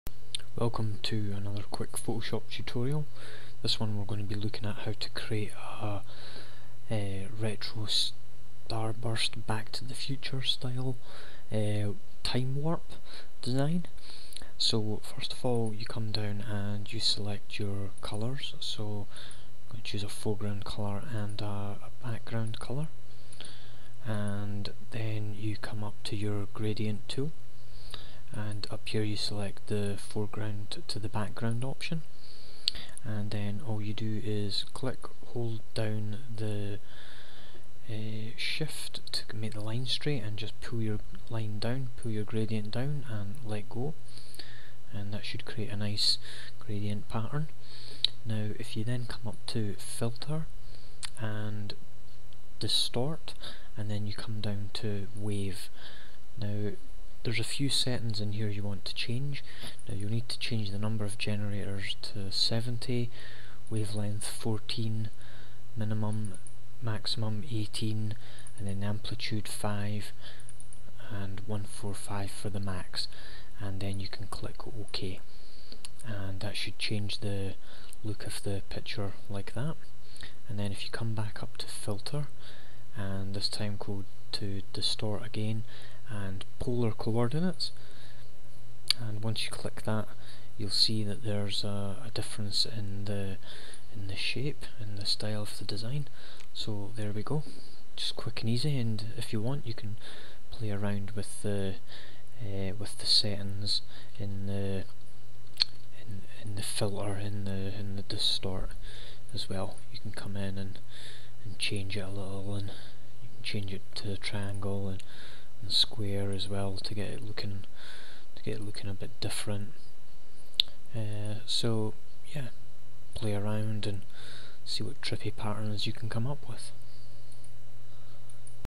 How to create a trippy timewarp pattern in photoshop
This is how to create a pattern of color in photoshop